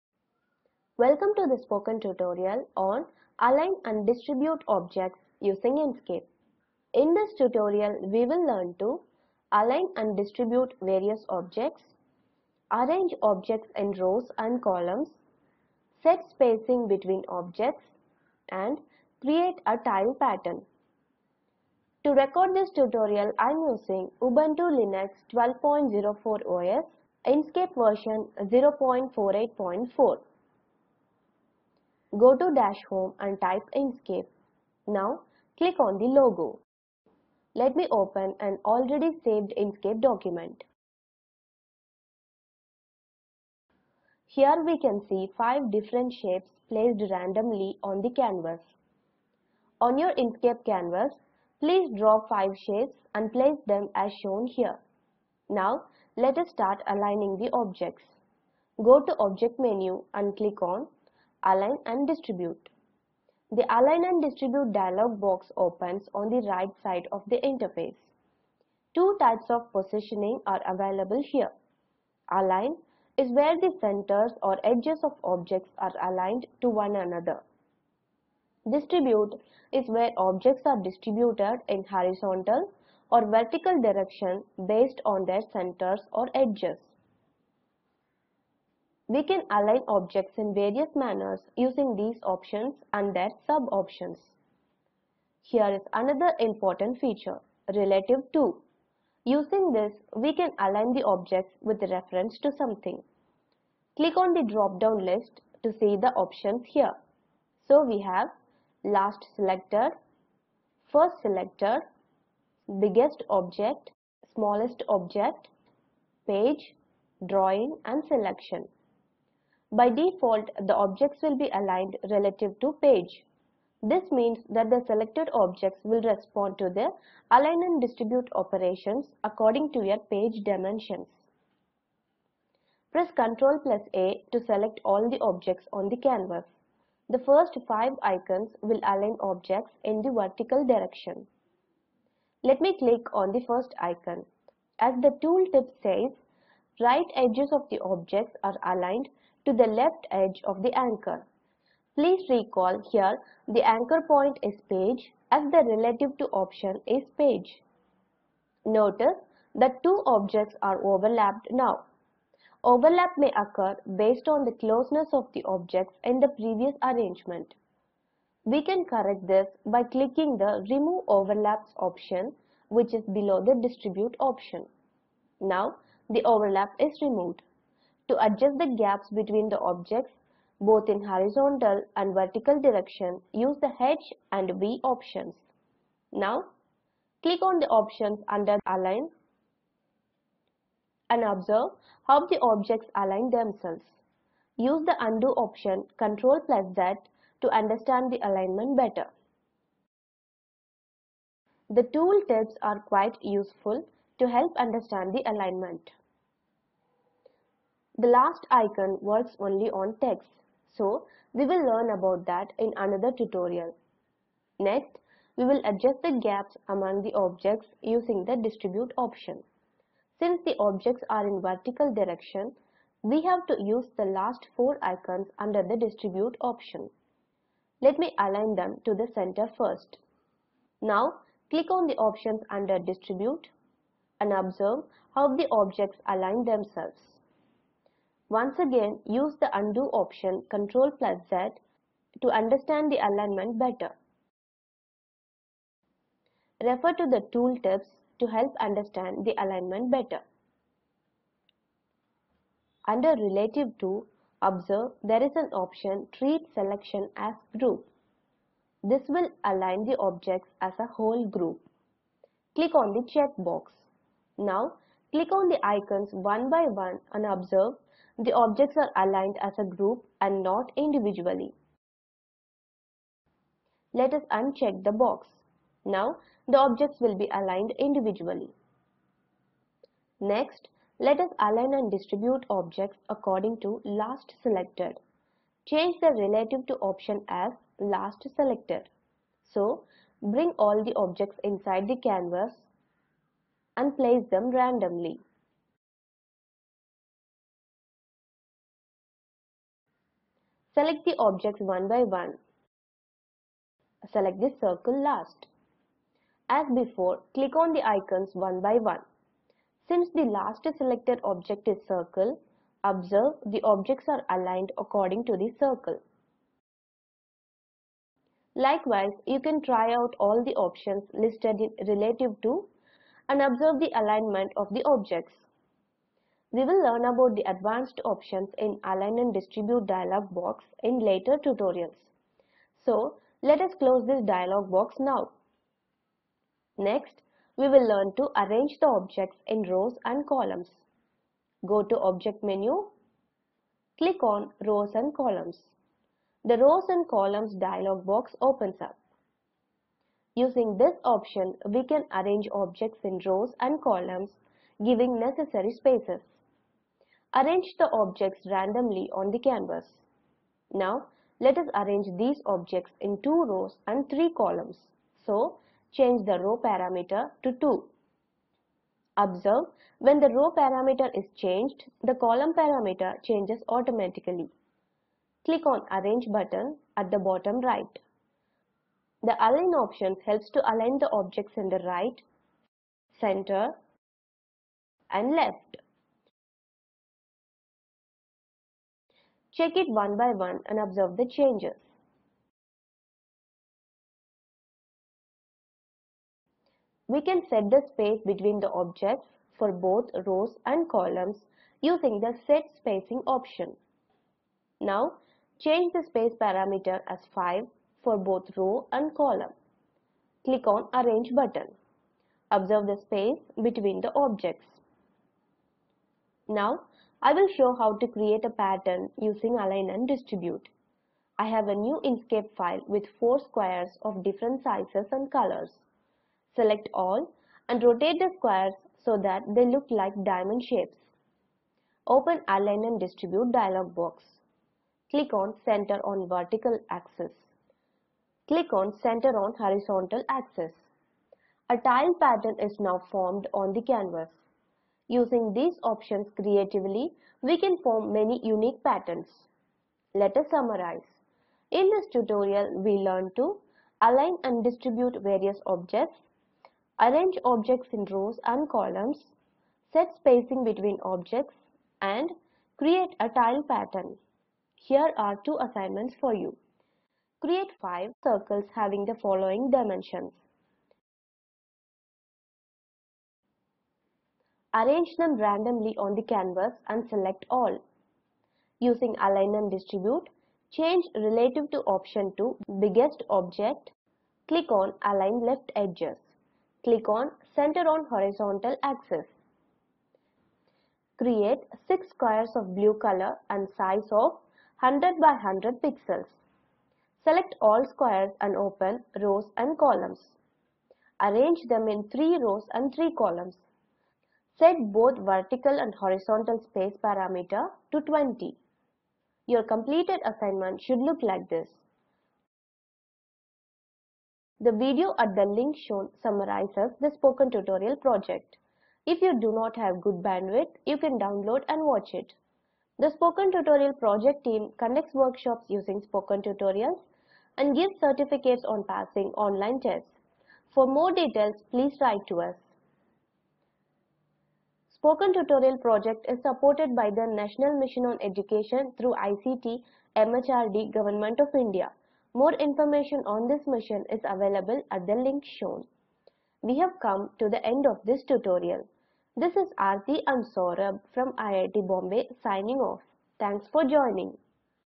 Align and distribute objects - English
*Align and distribute various objects
*Align objects with reference to something
*Arrange objects in rows and columns
*Set spacing between objects
*Create a tile pattern